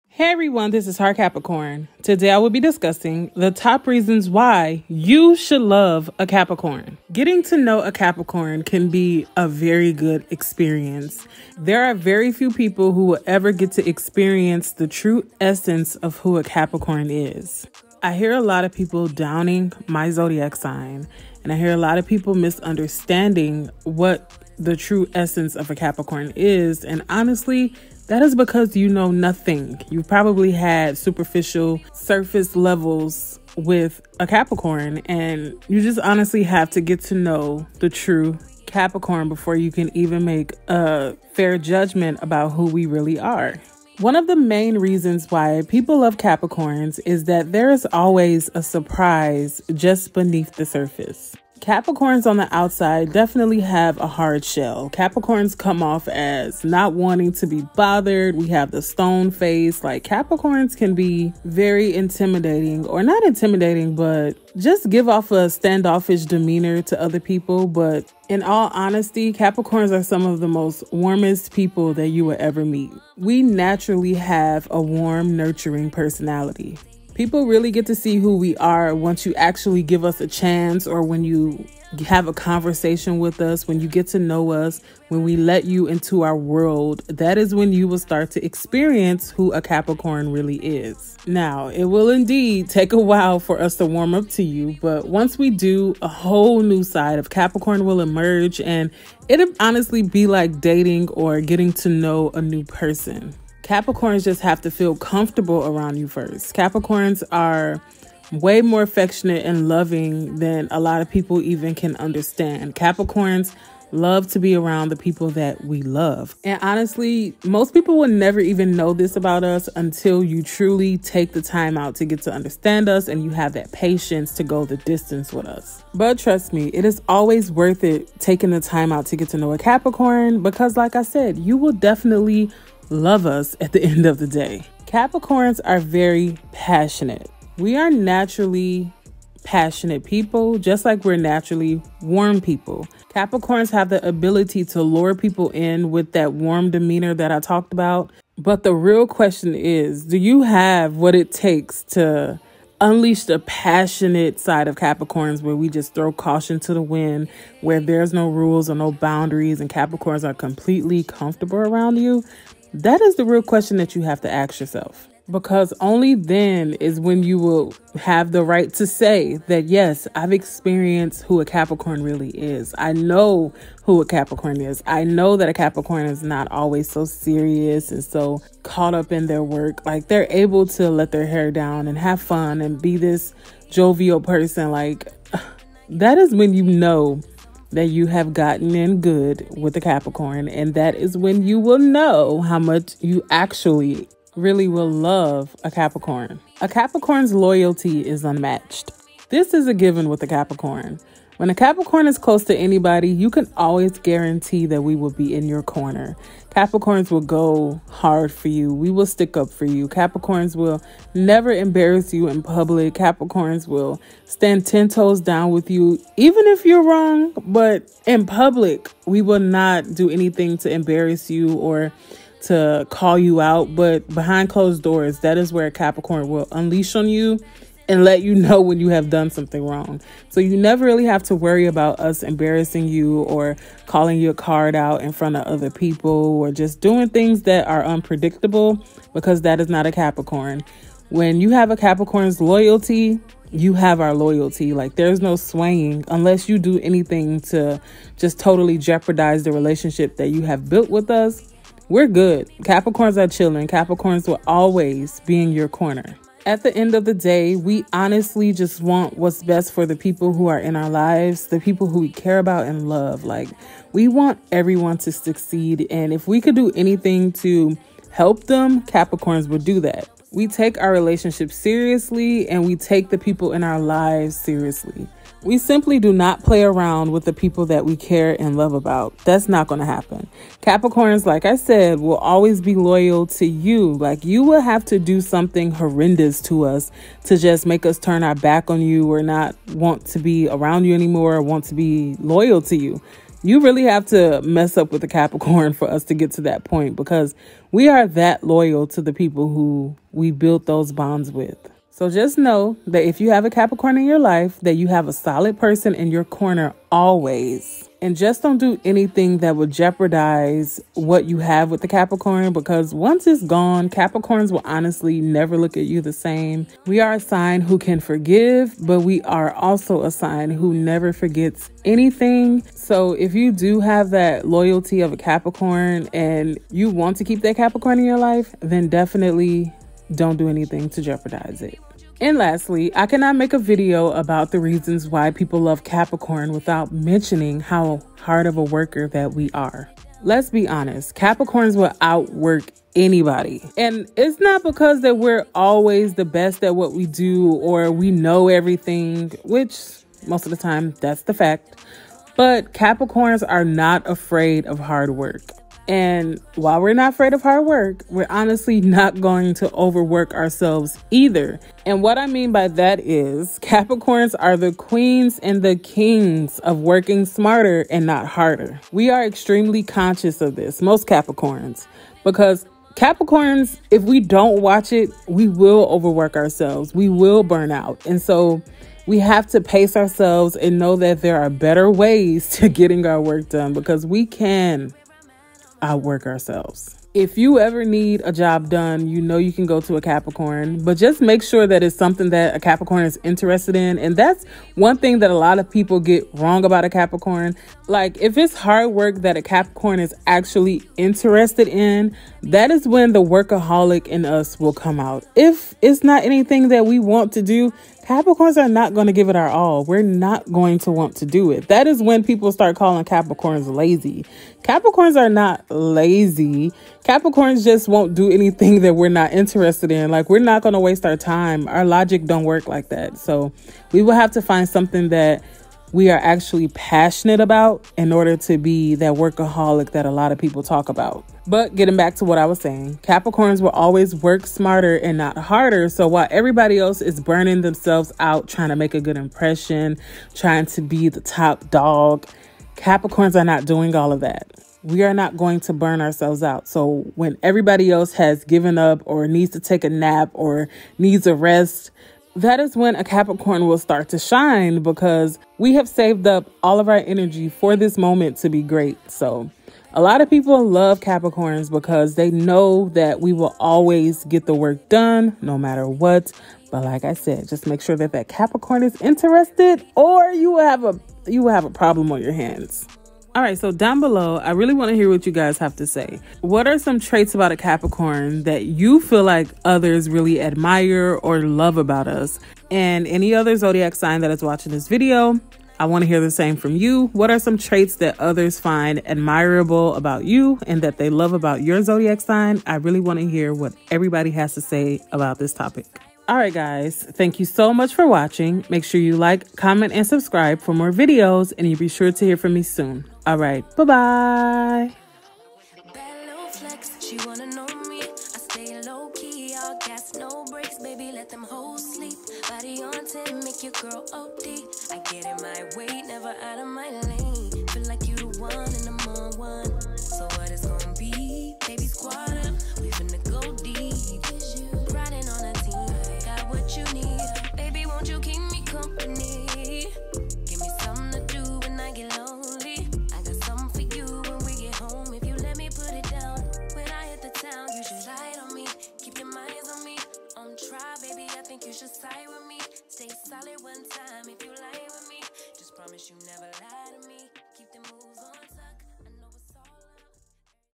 Top Reasons You Love Capricorns ❤️♑️
In this video I will be discussing Top Reasons You Love Capricorns? Enjoy❤️!

Please don’t take anything presented in this video personal. The video was made for entertainment/educational purposes only, not to personally attack you! This a generalized video based on Capricorn Sun Sign; I am aware that there are many factors that play a role in individual personalities (which I have not gone deep into YET). This is just a summary.